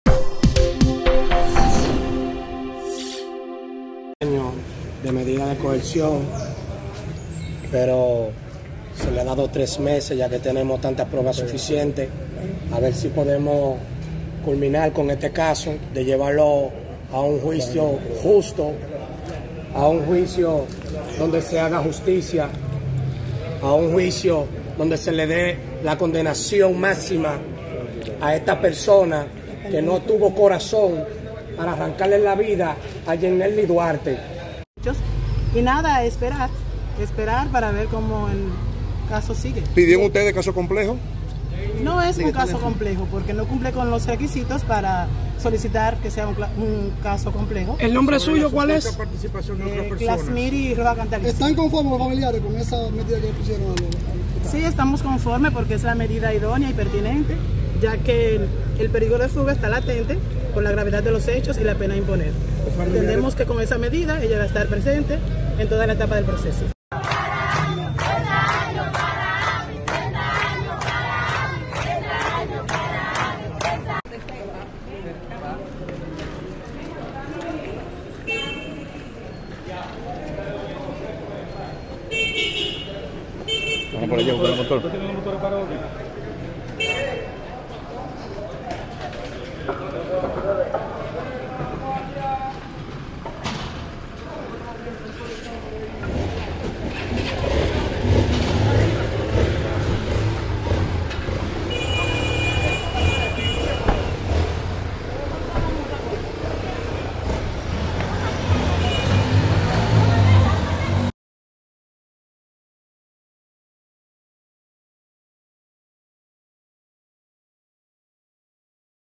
Imponen tres meses de prisión preventiva contra joven acusada de matar a Yenely Andreina
Tres meses de prisión preventiva fue la medida de coerción impuesta por el Tribunal de Servicios de Atención Permanente, contra la joven Ammy Hiraldo Peña, acusada de matar a Yenely Andreina Duarte.